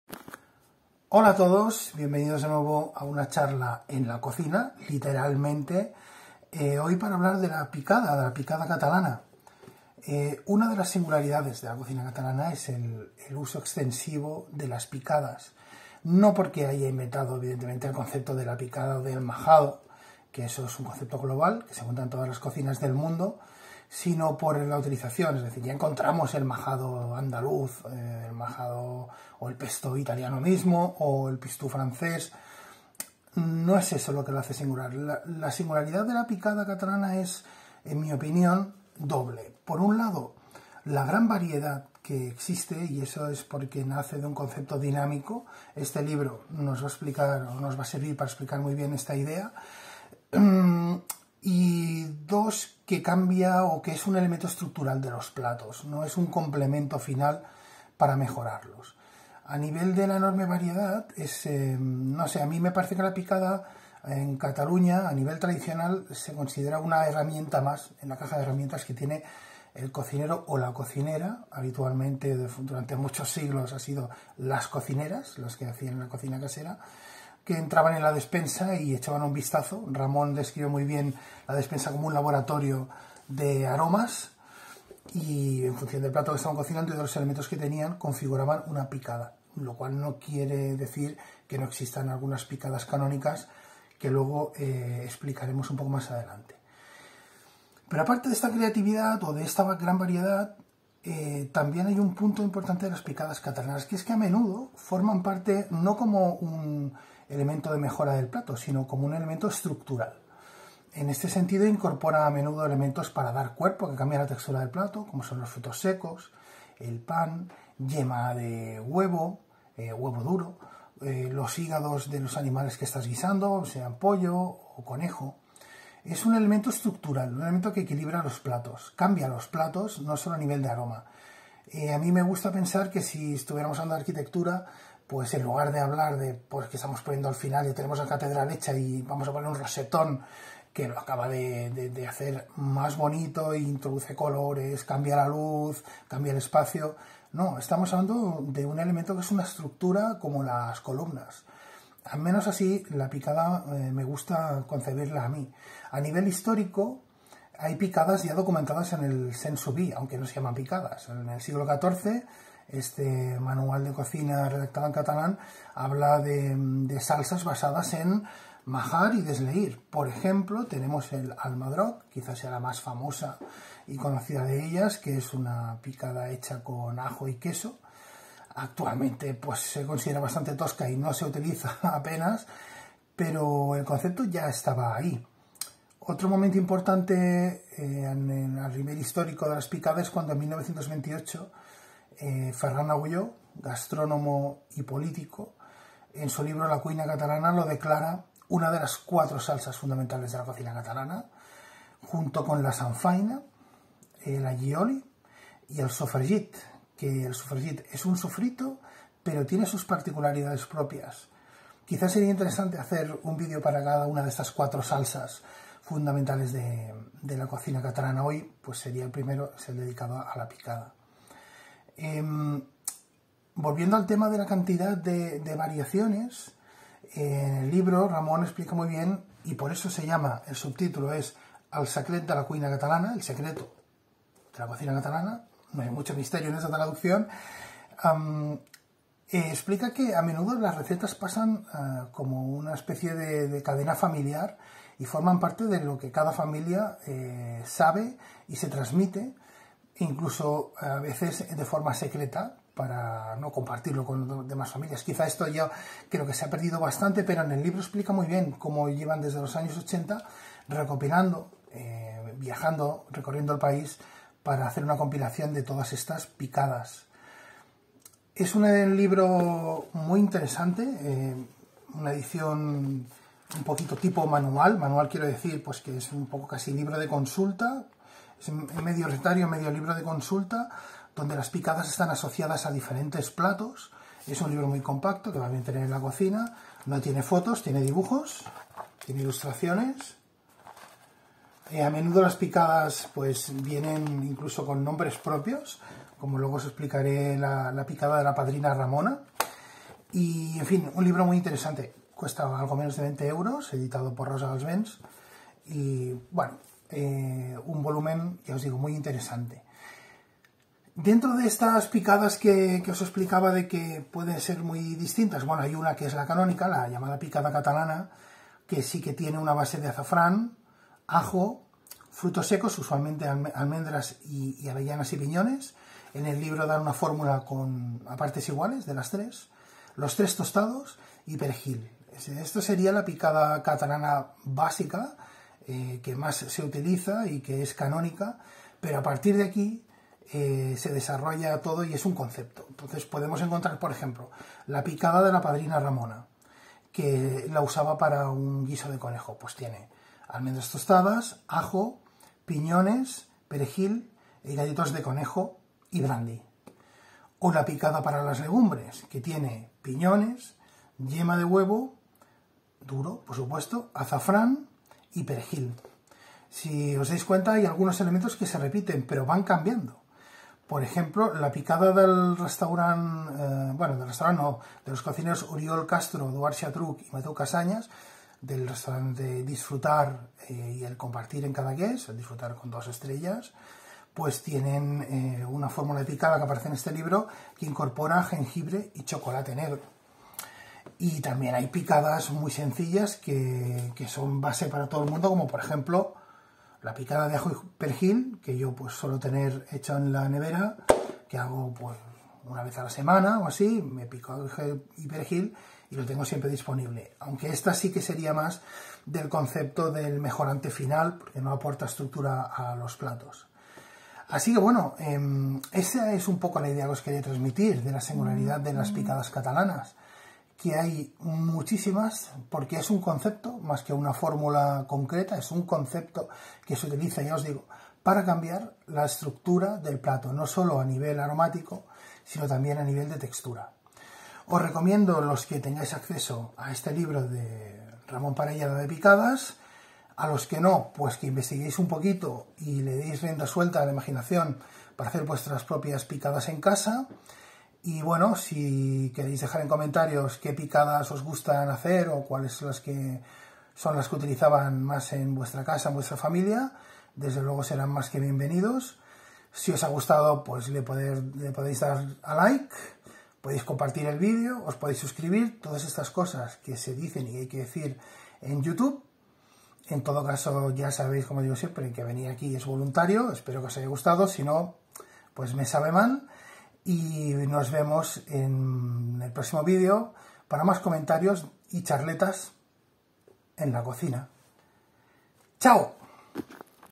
LA PICADA. El secreto de la cocina catalana.
La picada no solo embellece los platos en el paladar sino que los construye y sustenta aportando estructura, textura y complejidad.